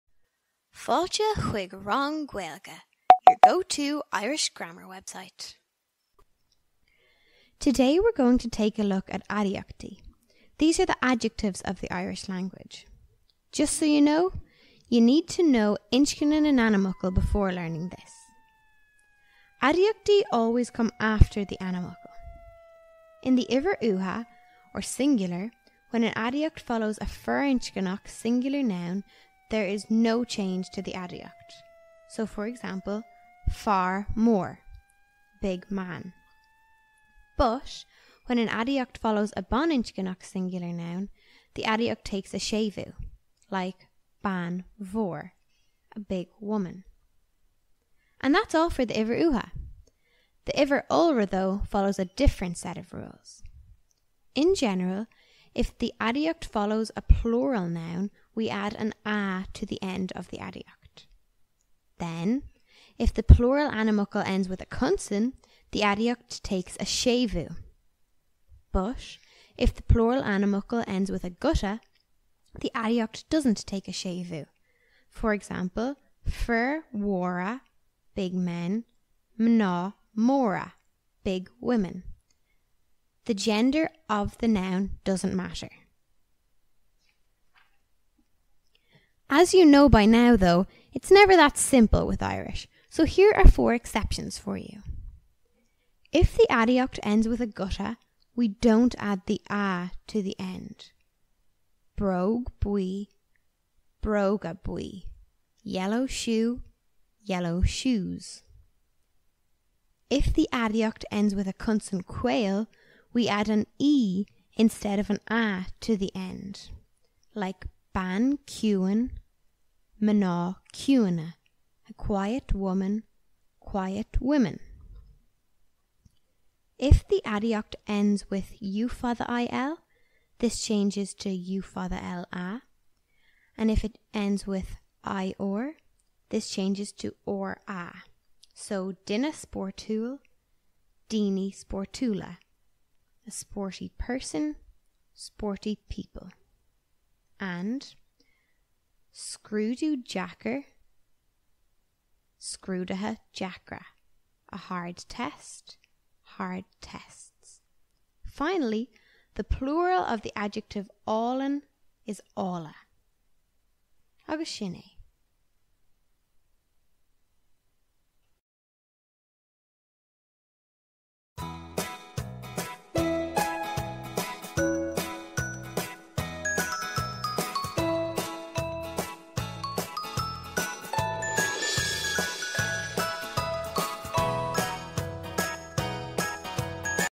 Aidiachtaí 1​ - Rang Gaeilge (NO MUSIC 2020)
Here you go! The non-techno version of our first video lesson on Aidiachtaí. to grab your free 10 point summary, practice worksheets and answer keys!

Ahh Aidiachtaí... Where would we be without these little describing words! How boring our language would be...
Just a heads up! In this video we will go through the rules of the Aidiachtaí. It is important to note that you will need some knowledge of:
Na Téarmaí Gramadaí
Inscne na n-ainmfhocal

So feel free to flick back over the videos for those if you need to!